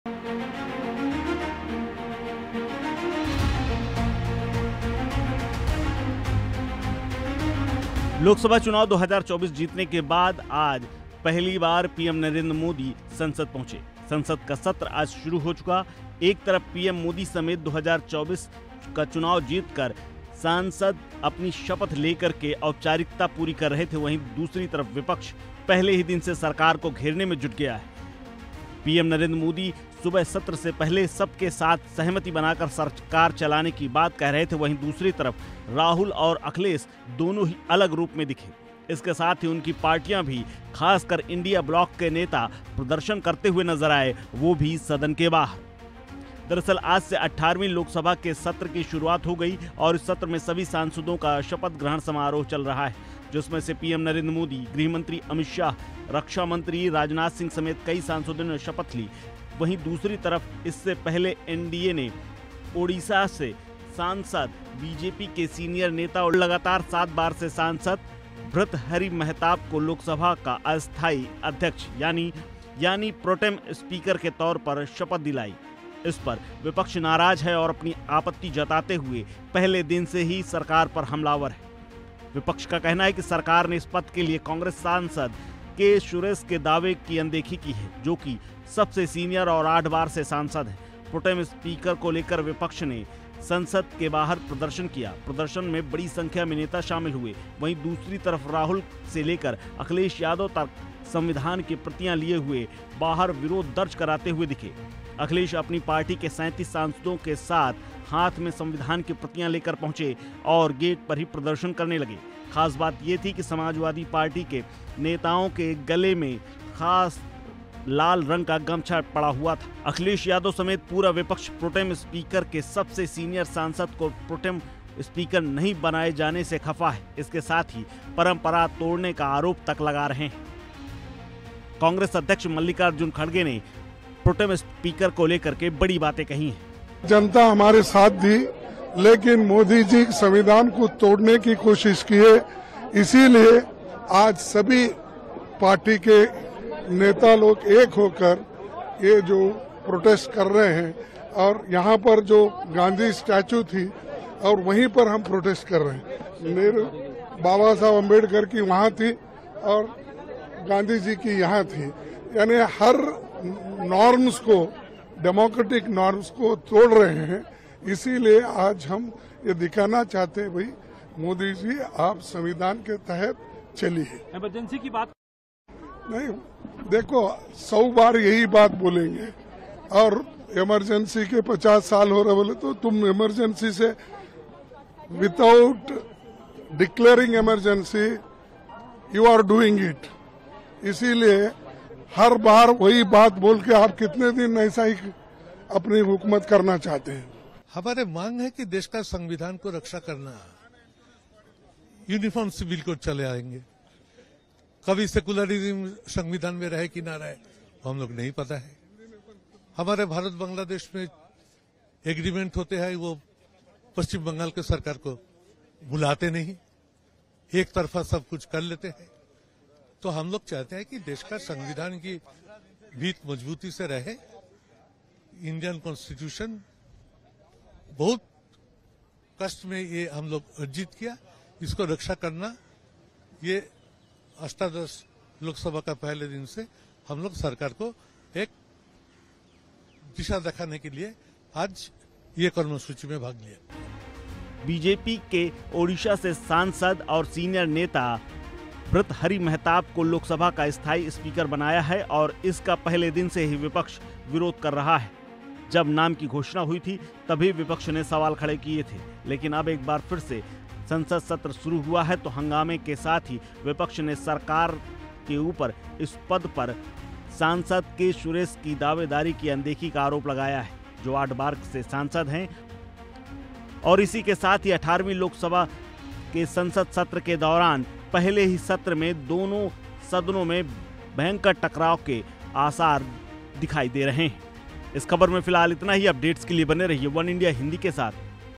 Parliament Session 2024: PM Modi ने ली शपथ, Rahul Gandhi और Akhilesh Yadav बाहर रहे |वनइंडिया हिंदी
आज से 18वीं लोकसभा (Loksabha Session) के सत्र की शुरुआत हो गई है. इस सत्र में सभी सांसदों का शपथ ग्रहण समारोह हो रहा है। जिनमें से प्रधानमंत्री नरेंद्र मोदी (PM Modi), गृहमंत्री अमित शाह (Amit Shah), रक्षा मंत्री राजनाथ सिंह (Rajnath Singh) सहित कई सांसदों ने शपथ ले ली है।